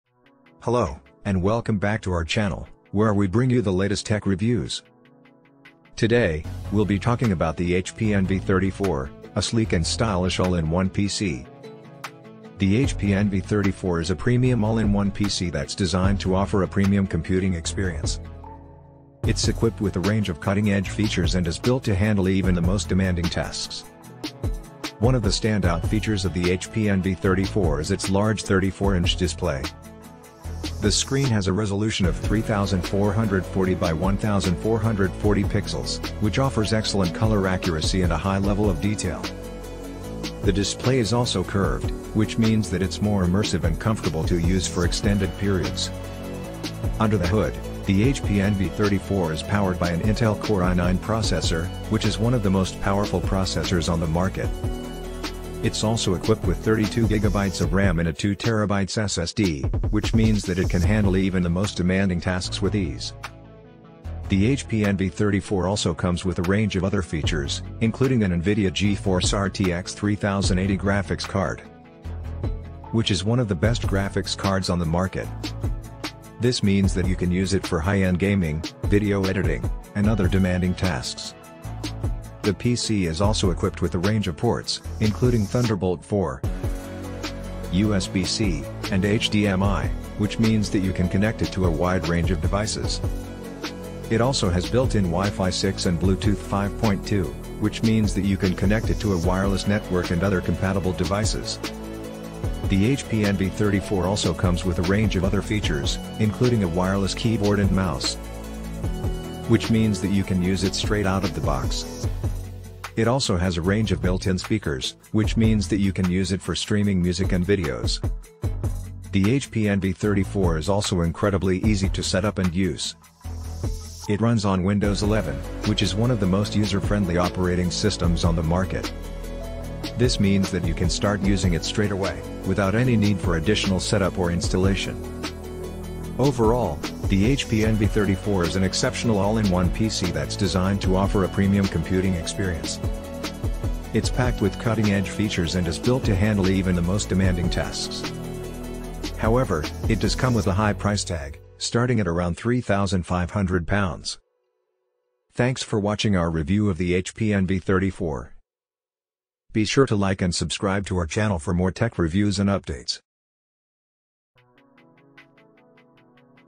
HP Envy 34 (2022): The Ultimate Curved All-in-One Review
Amazon Product Name : HP Envy 34 (2022)

The HP Envy 34 (2022) is a stunning all-in-one desktop computer that boasts a sleek design and impressive performance. In this review, we take a closer look at its features, including its 34-inch curved display, powerful Intel processor, and immersive audio system. We also explore its user experience, including its ease of use and customization options.